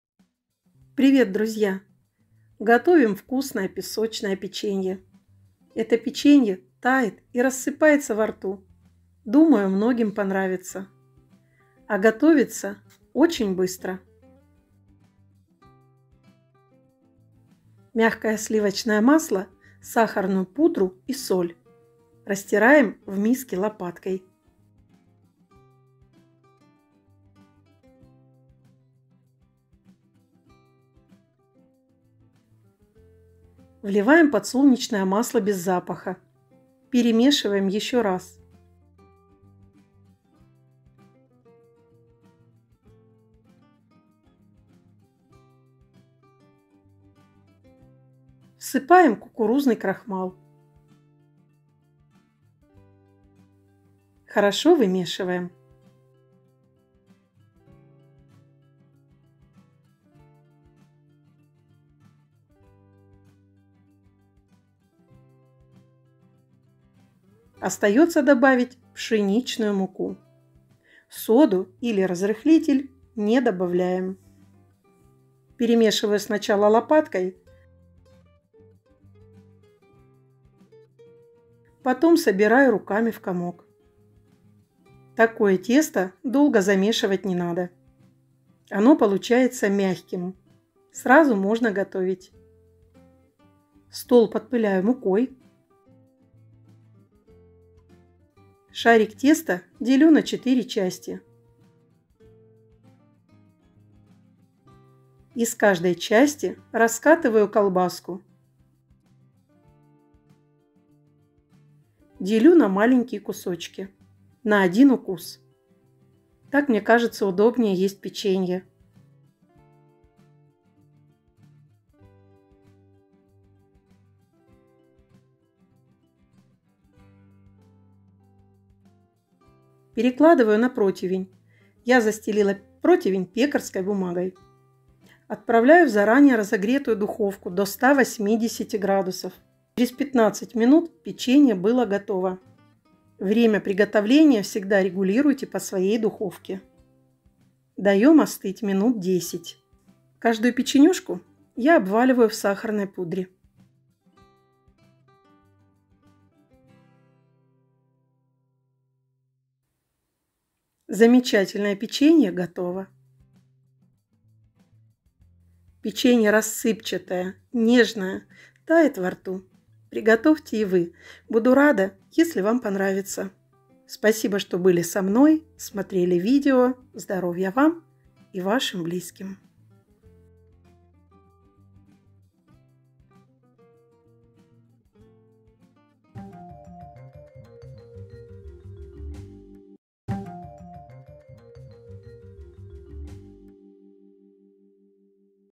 Вкусное,рассыпчатое печенье. ПЕЧЕНЬЕ ХРУСТАЛИК.
Друзья, всем большой привет
Предлагаю приготовить вкуснейшее печенье
Такое печенье тает во рту
Получается рассыпчатое,нежнейшее
песочное печенье
Рецепт:
включаем духовку на 180 гр
сливочное масло 100 гр(мягкое)
сахарная пудра 80
соль щепотка
растираем лопаткой
Вливаем подсолнечное масло 100 мл
все перемешаем
Всыпаем кукурузный крахмал 100 гр
размешиваем
Всыпаем пшеничную муку 200 гр
вымешивать долго не надо
Собрали в комок и тесто готово
Делим на 4 части
каждую раскатываем в жгут
делим на кусочки
Перекладываем на противень
Выпечка 12-15 минут
Даем остыть минут 10
Каждое печенье обваливаем в пудре
Готово
Приятного чаепития